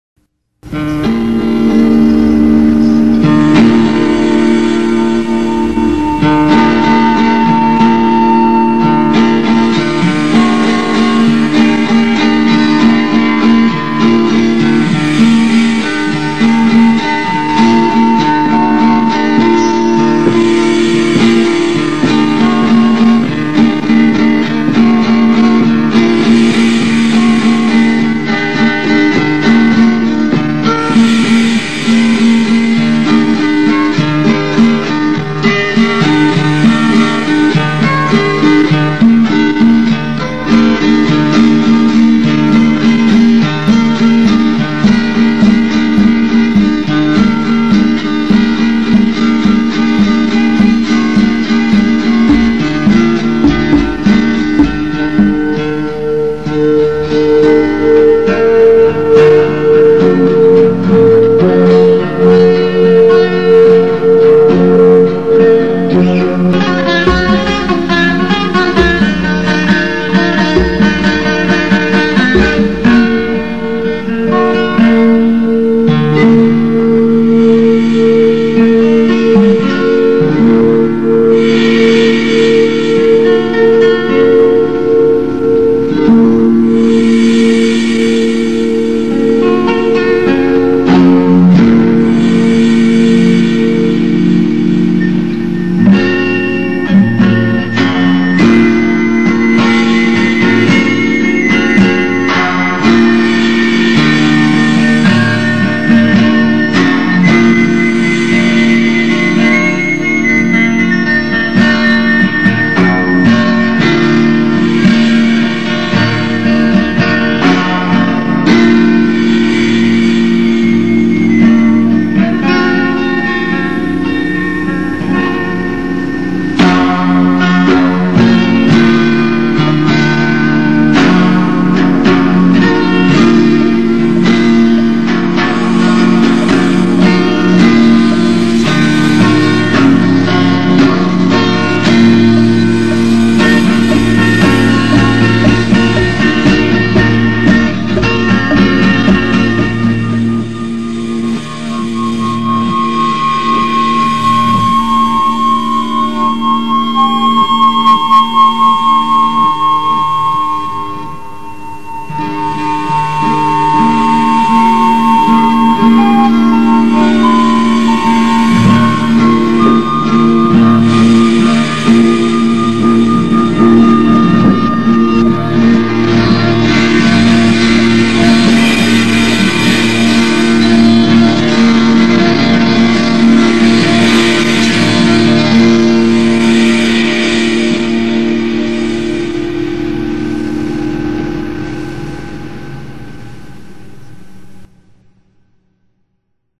Drone
background music experiment i wrote in the 1990's - recorded on a 4 track recorder and bounced on to a cassette tape

film public domain NASA footage